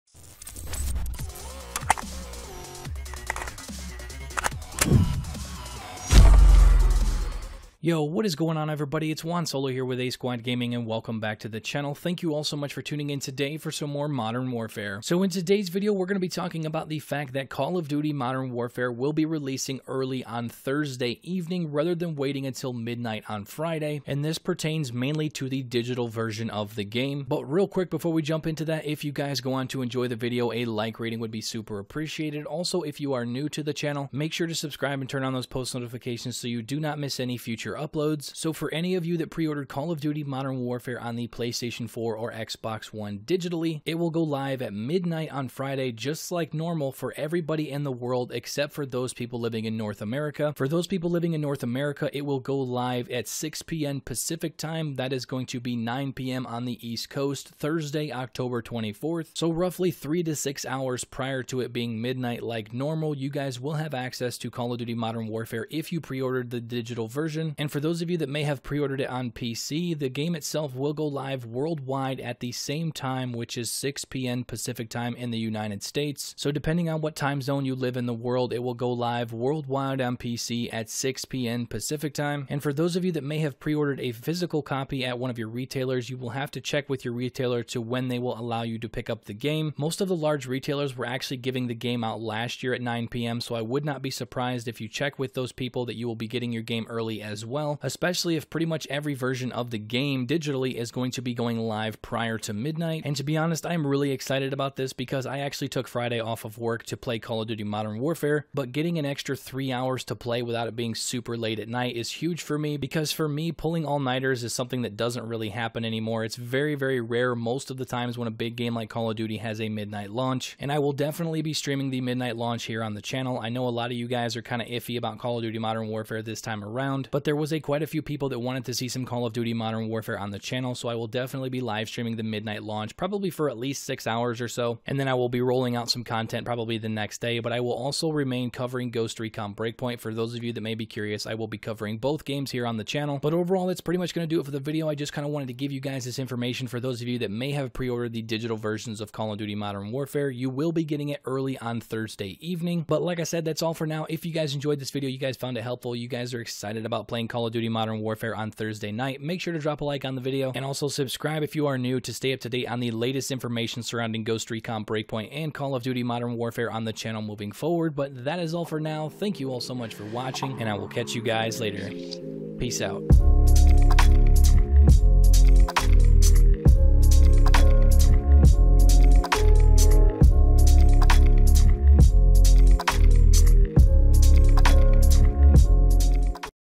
Call of Duty Modern Warfare | Early Release! Play Early on Thursday!
In this video I go over all of the info surrounding the Early Release of Call of Duty Modern Warfare! I hope you enjoy!